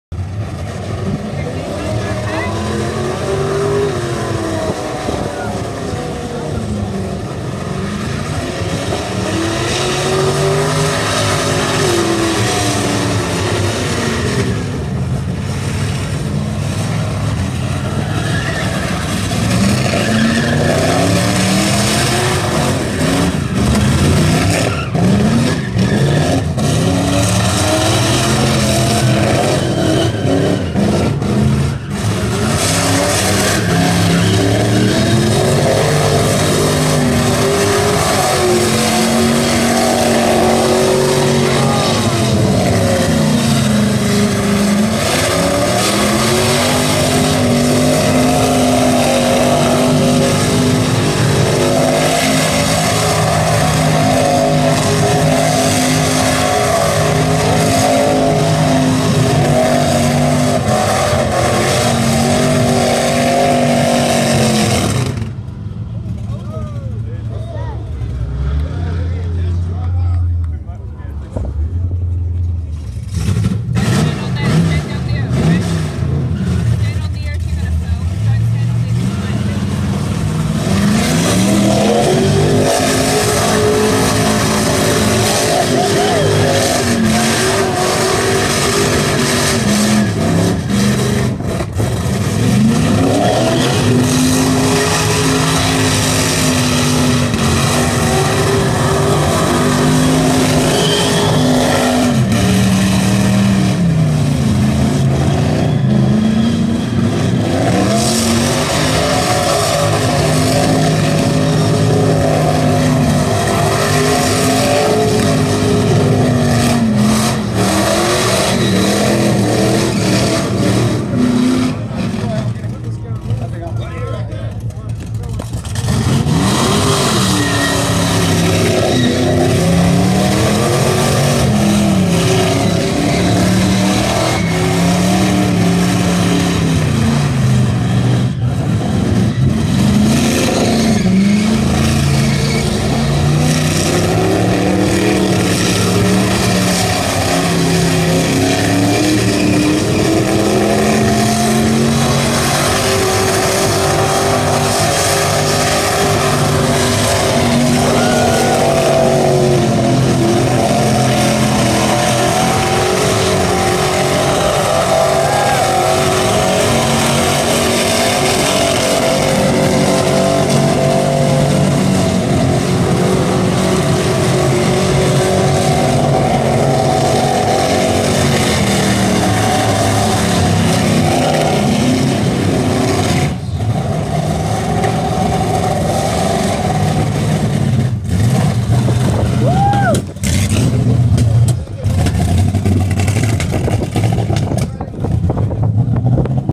H8EVOS 1st skid on a pad V8 chev powered WRX STi RA
1st time on the pad, wasn't running right which turned out to be spark scatter from cracked dizzy rotor and it just wouldn't sing in second gear. Gave the cooling system a good test though!